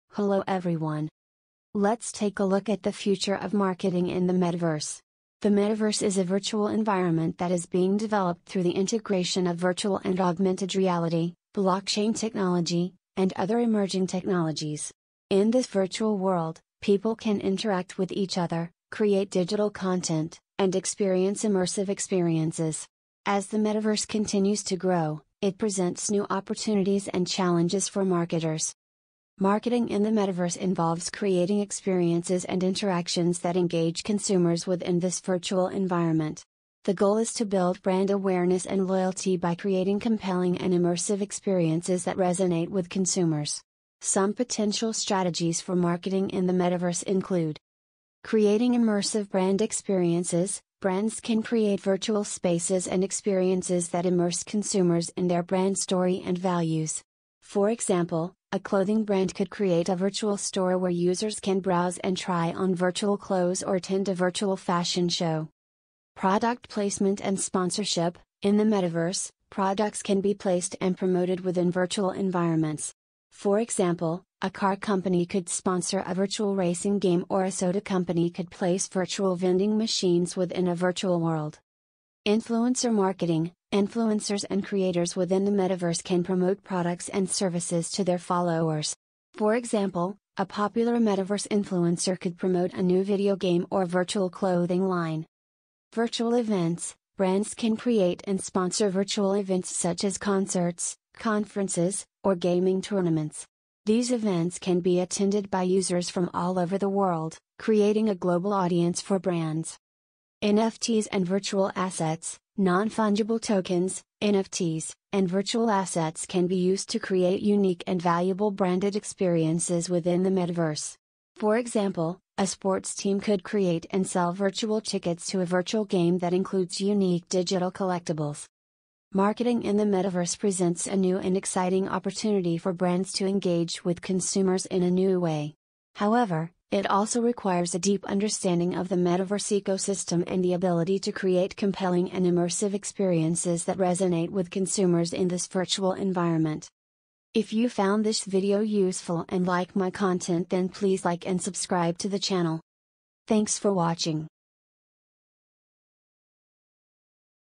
Marketing in the Metaverse
Marketing in the metaverse involves creating experiences and interactions that engage consumers within this virtual environment. The goal is to build brand awareness and loyalty by creating compelling and immersive experiences that resonate with consumers.